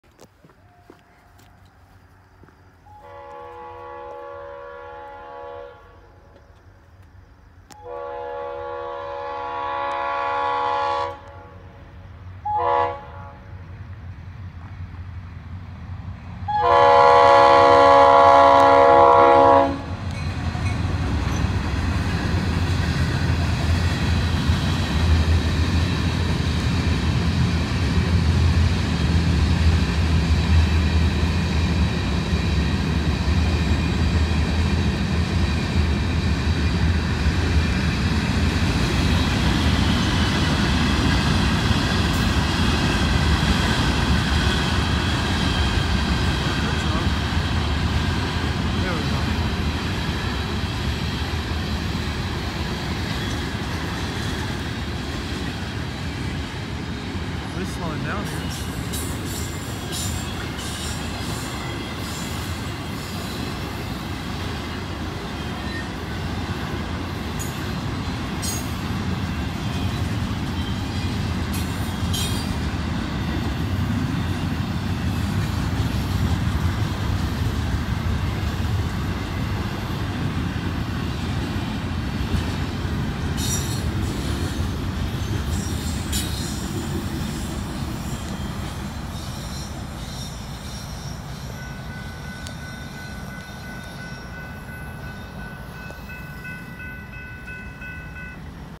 FEC 950 With The 418 In Hobe Sound... Again!
Right after 105 came 950! Pretty cool seeing the 418 again.

Filmed on December 2, 2021, at 6:49PM.